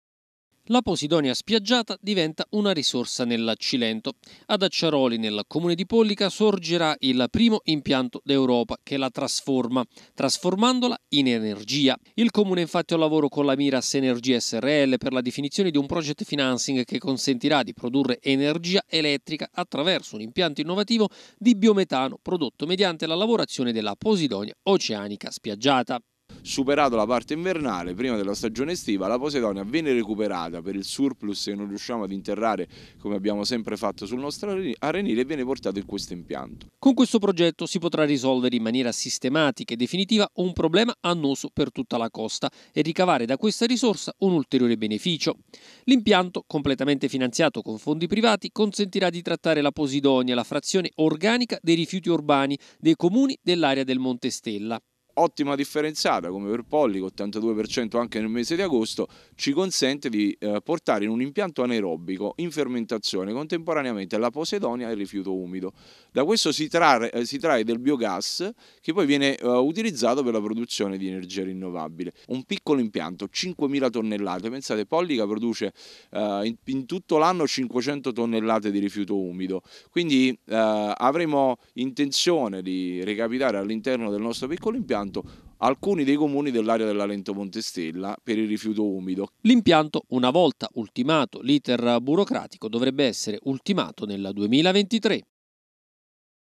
Pollica, energia grazie alla posidonia spiaggiata
Il sindaco Pisani: "Primo impianto in Europa"